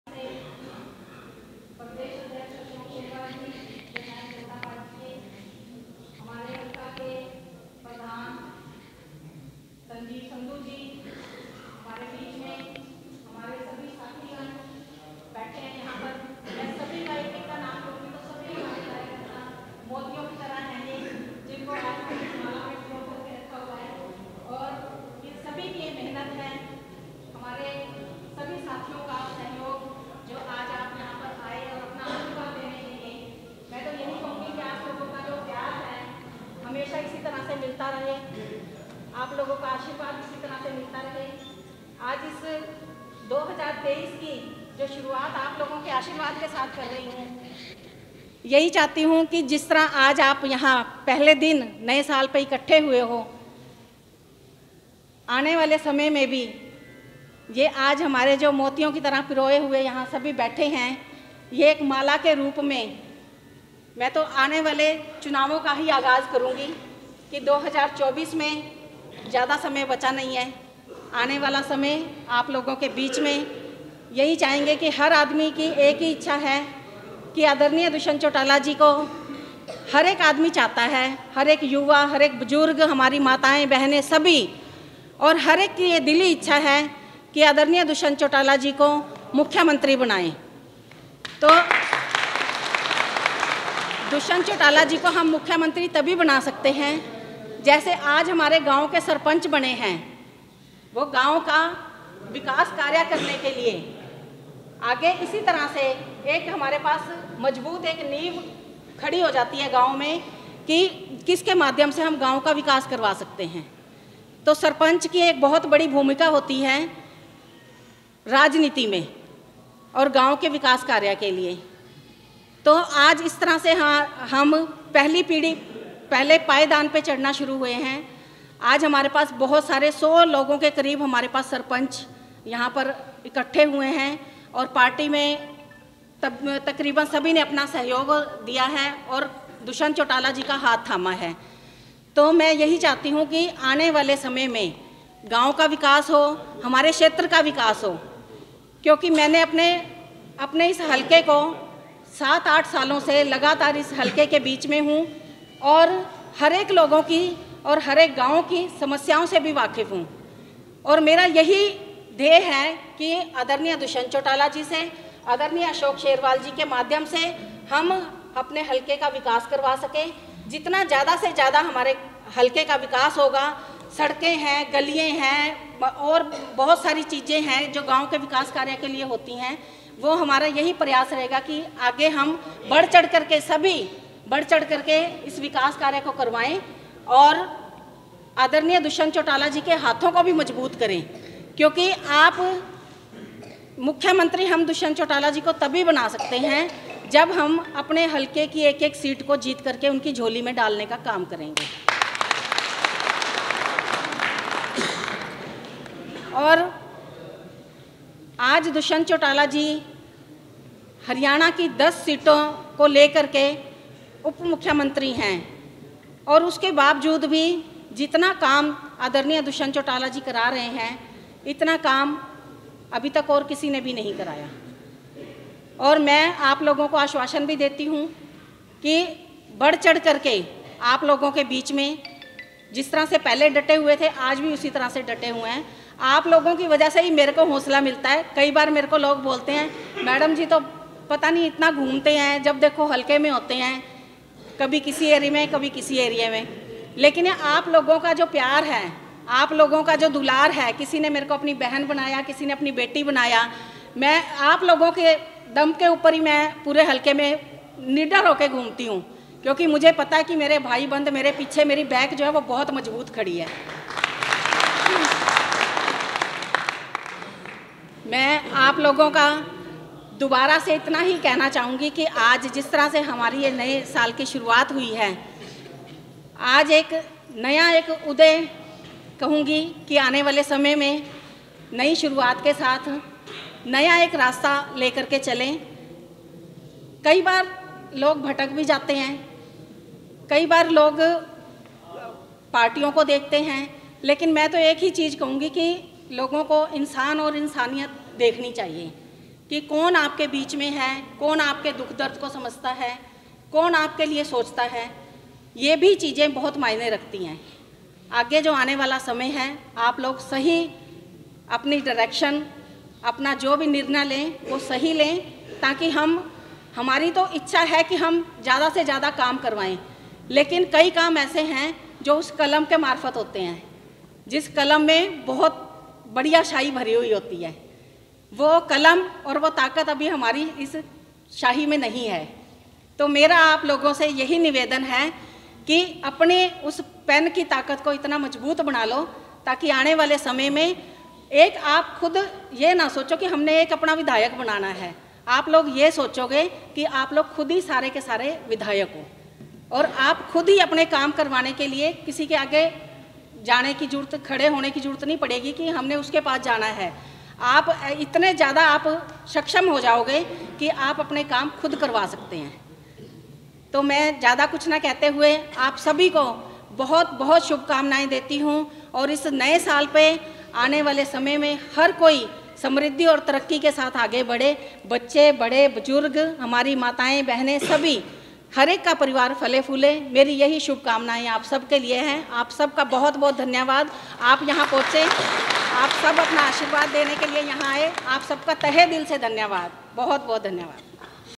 V-758.पार्ट 3-7.जननायक जनता पार्टी हल्का सधौरा के कार्यकर्ताओं का नव वर्ष मिलन समारोह 1 जनवरी 2023.
विडिओ -758.
पार्ट 3-7.
     जननायक जनता पार्टी हरयाणा हल्का सधौरा के कार्यकर्ताओं द्वारा नव वर्ष मिलन समारोह 1 जनवरी 2023 का आयोजन किआ गया जिसमें श्री अशोक शेरवाल प्रदेश अध्यक्ष जननायक जनता पार्टी, डा. कुसुम शेरवाल प्रदेश महां सचिव व प्रवक्ता जननायक जनता पार्टी हरयाणा, श्री संजीव संधू अध्यक्ष हल्का साधोरा जननायक जनता पार्टी व बहुत से कर्ताओं ने भाग लिआ!